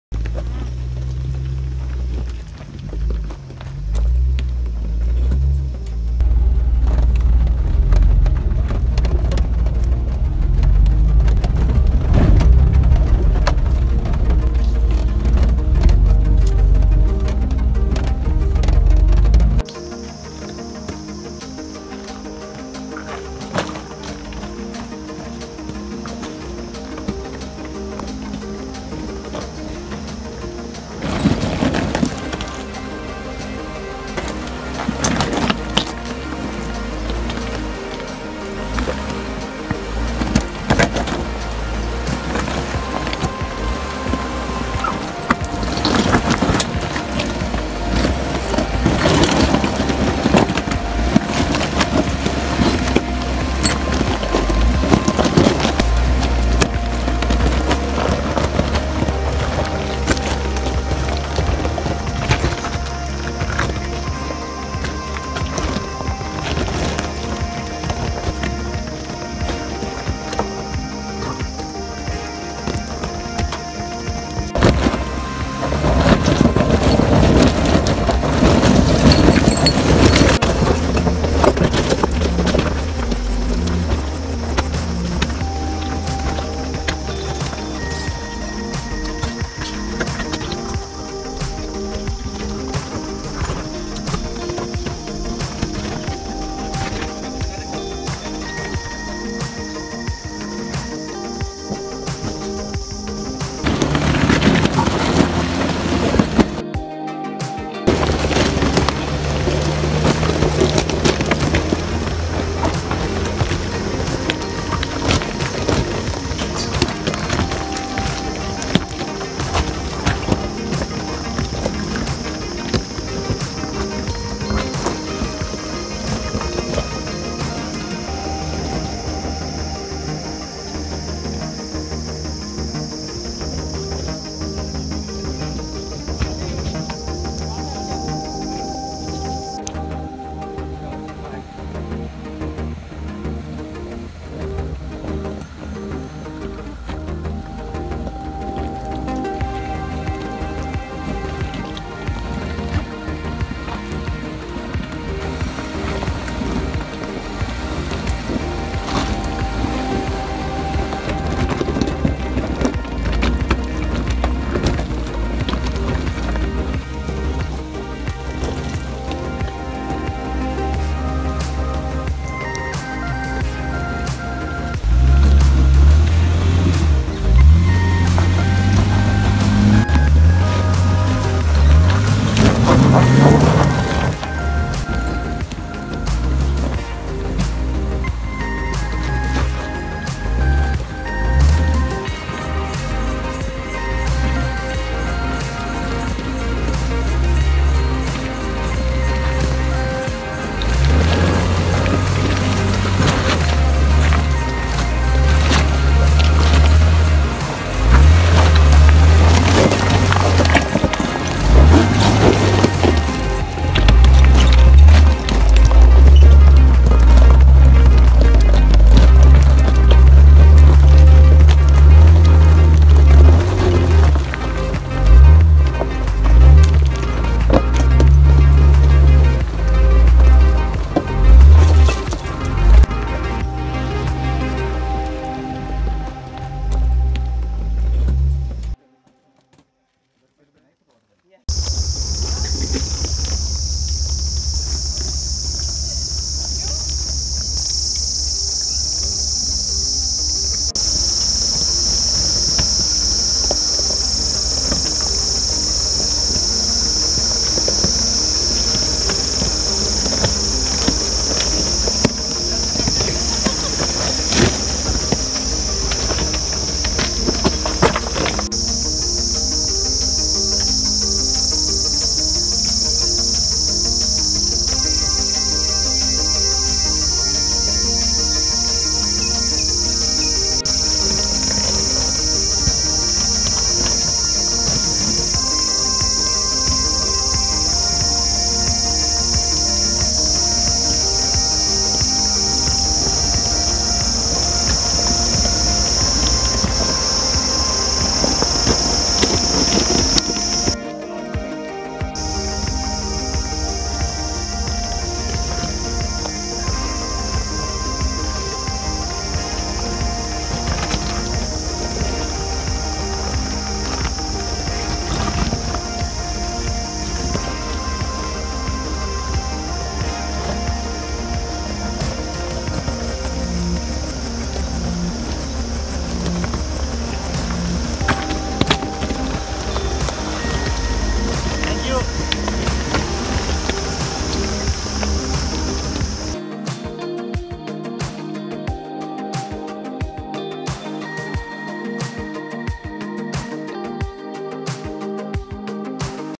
Oman Off-road - Yitti Village
Adventure Oman team off-roading at Yitti Village hills